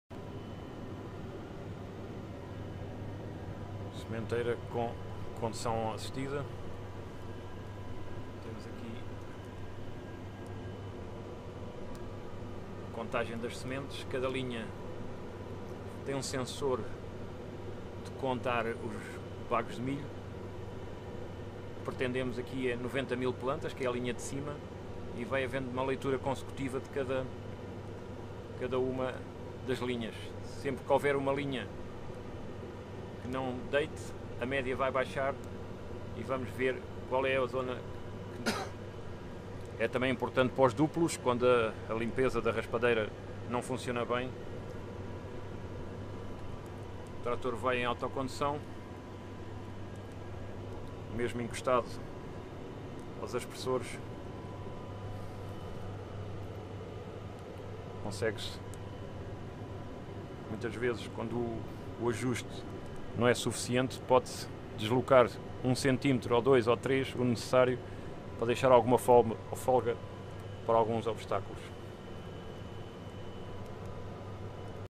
Sementeira Milho 2018 (AP Condução Assistida+Contagem de Sementes) (translated subtitles)
Demos inicio às sementeiras 2018 no dia 18 Abril
As condições não são as melhores, mas já temos o solo com boa temperatura e os solos arenosos drenaram muito bem e conseguimos encontrar um equilíbrio aceitável entre as zonas um pouco mais pesadas e as zonas arenosas.
Tentamos dar algum espaço de tempo entre a última mobilização e a sementeira para poder secar à superficie o solo para assim semear melhor . Tentámos não deixar  o sulco da sementeira aberto nos locais mais pesados.
A condução assistida permite uma perfeita sementeira mesmo nas coberturas totais onde o rigor é fundamental devido aos obstáculos do sistema de rega.
Garantir a contagem de semente aplicada durante a sementeira é fundamental para a garantia de uma boa distribuição espacial, sem falhas e sem duplos.

Acompanhe a temperatura do solo num campo agricola na Golegã